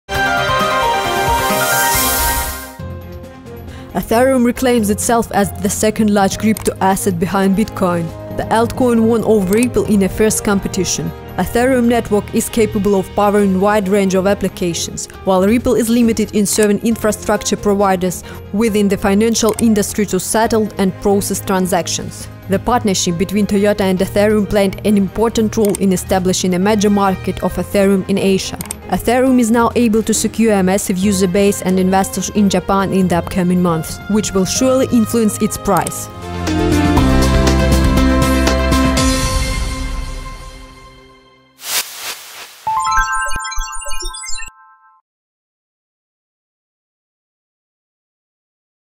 KCN Ethereum renews as second largest cryptocurrency
Ethereum reclaims itself as the second largest crypto asset behind bitcoin. The altcoin won over Ripple in a fierce competition. Ethereum network is capable of powering wide range of applications, while Ripple is limited in serving infrastructure providers within the financial industry to settle and process transactions. The partnership between Toyota and Ethereum played an important role in establishing a major market for Ethereum in Asia. Ethereum is now able to secure a massive user base and investors in Japan in the upcoming months, which will surely influence its price.